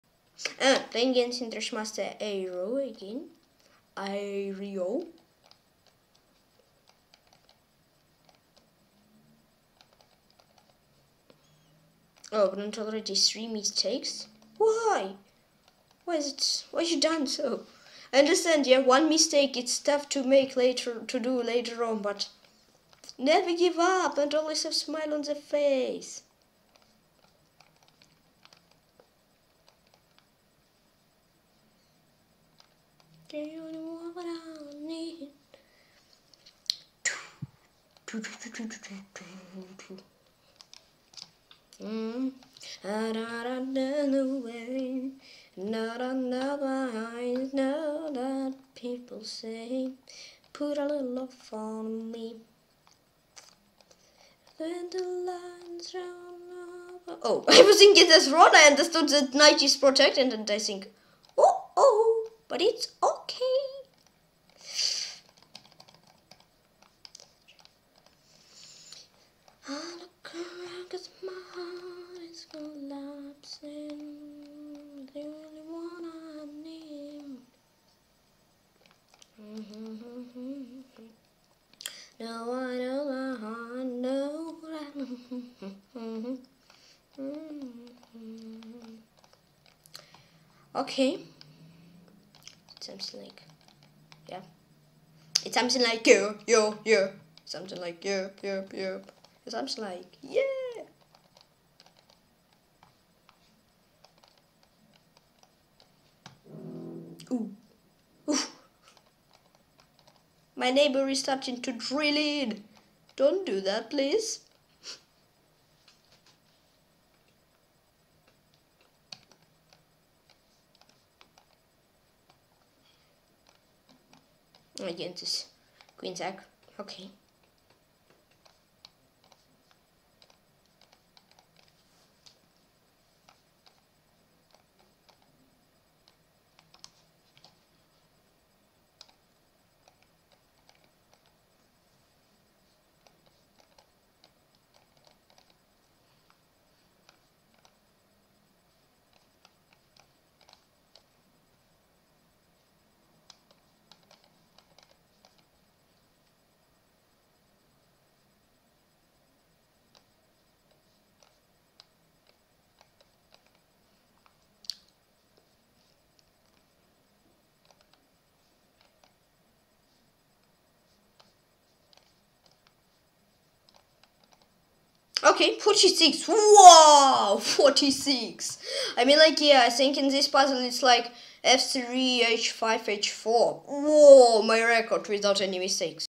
UNBELIEVABLE RECORD online!! Chess prodigy Tykhon(9 y.o.) did 46 in Puzzle Rush in 3 min. 23/12/2019
On stream Tihon decided 46 puzzles in 3 minutes and he did not make any mistakes!